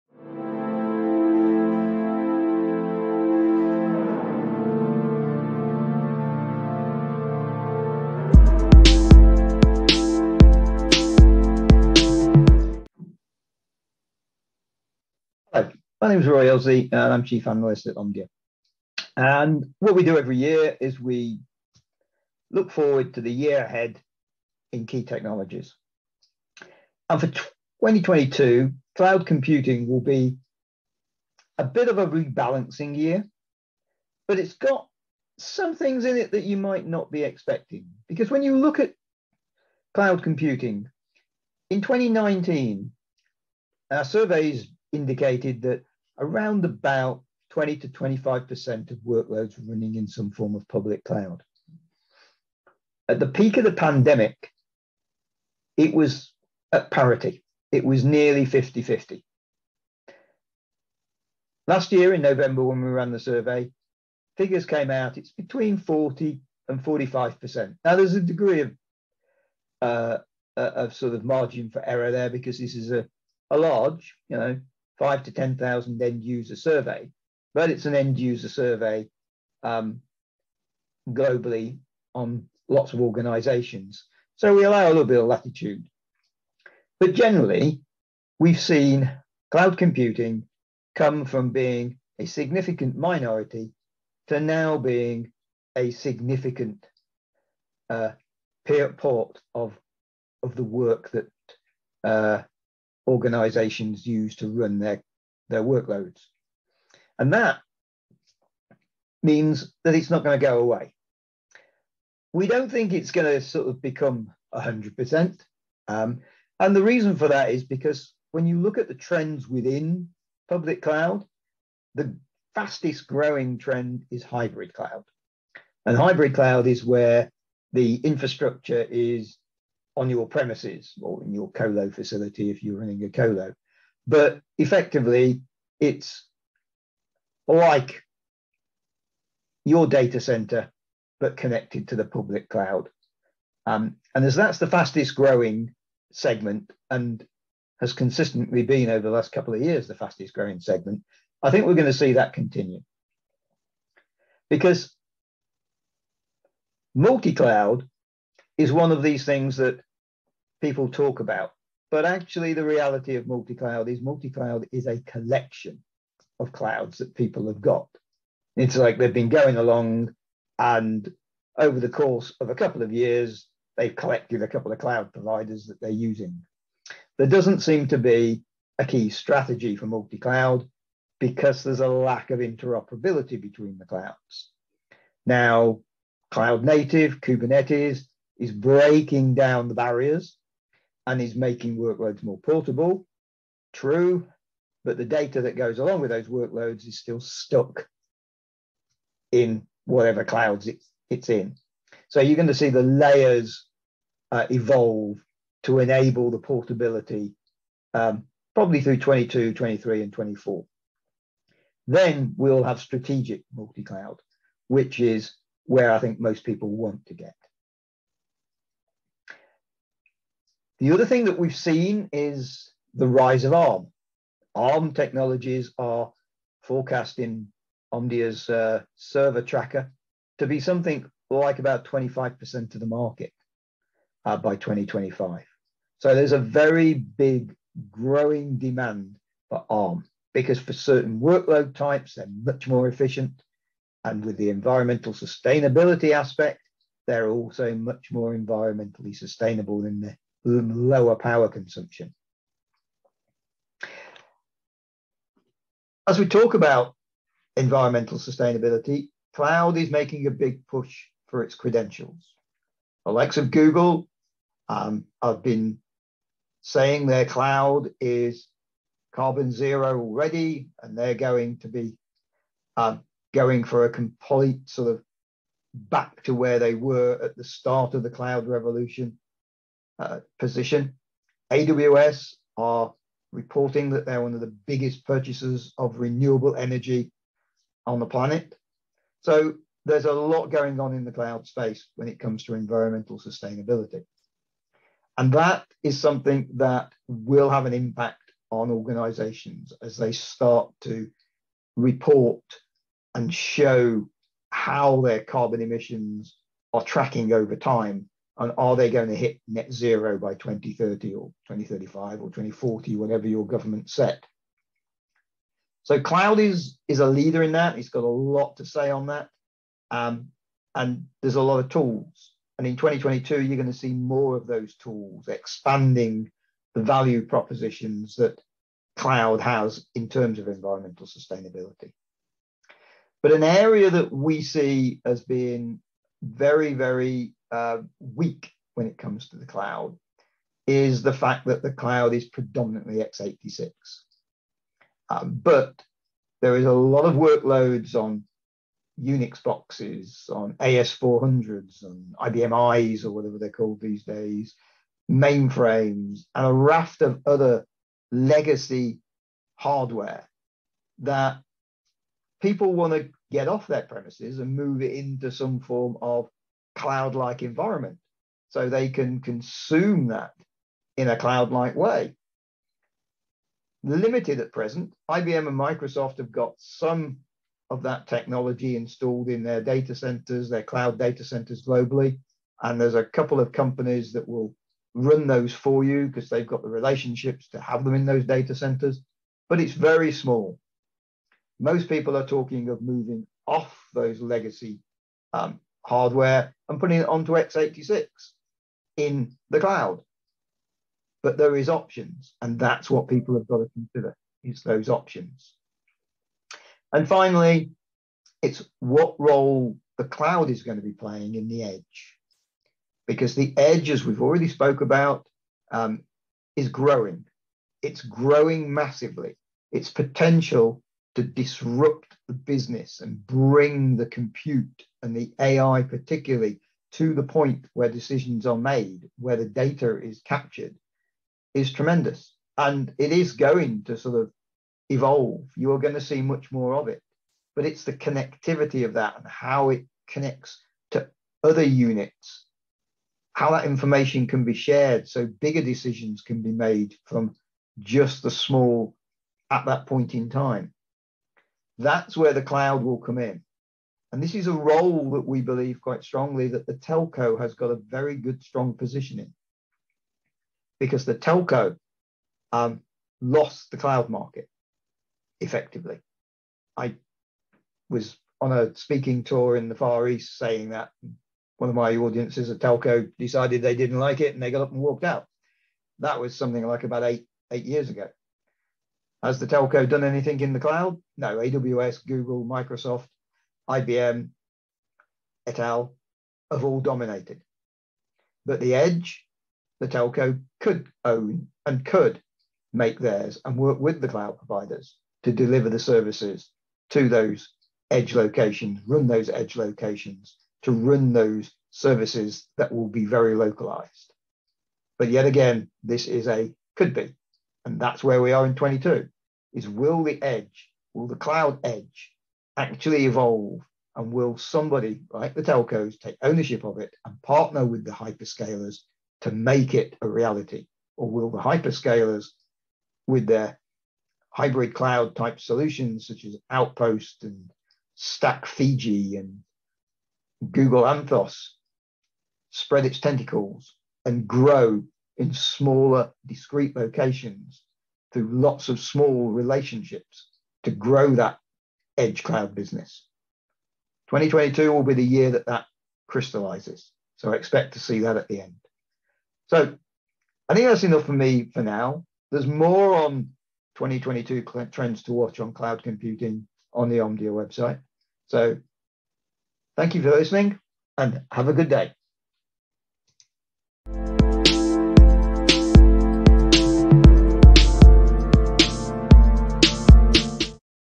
2022 Cloud Computing Trends
Omdia chief analyst Roy Illsley discusses the top cloud computing trends of 2022.